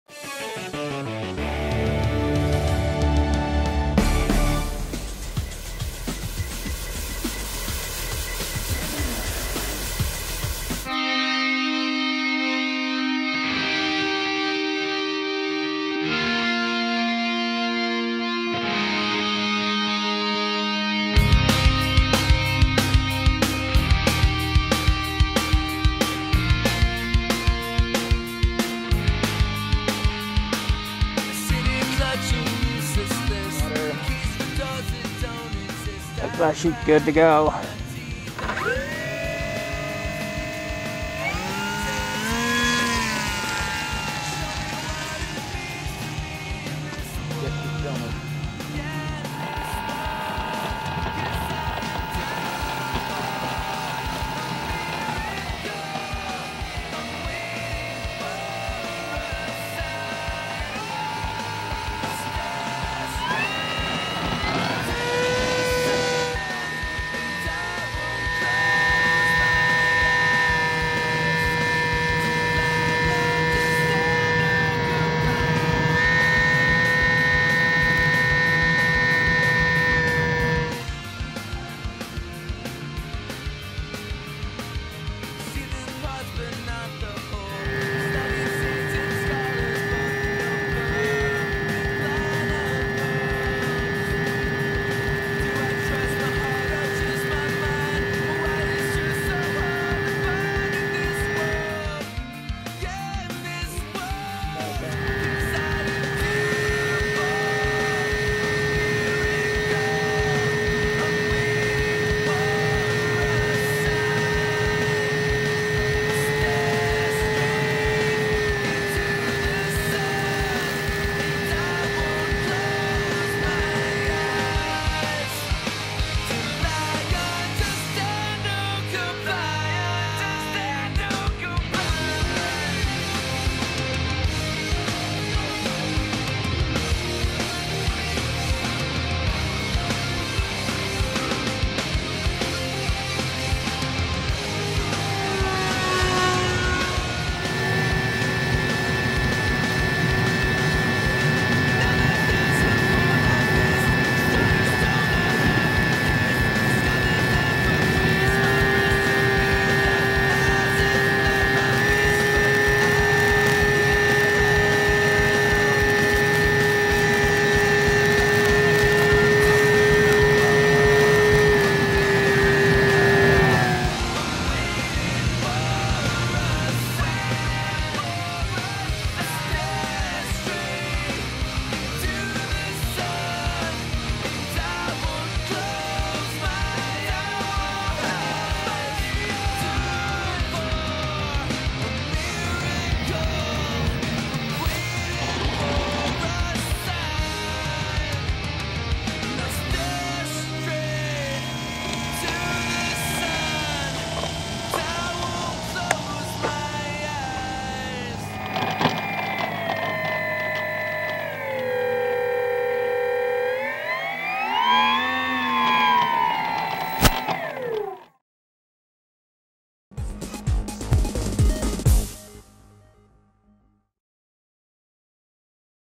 HobbyKings HK FPV 168 Long Range Plane
For set-up and build info visit: FPVpilot.com in the project pages. My Maiden flight of the Hobbyking FPV 168 Fiberglass plane. Flies smooth and is strong. Here I'm testing it loaded up at RTF weight of 6 lbs/2.70kg.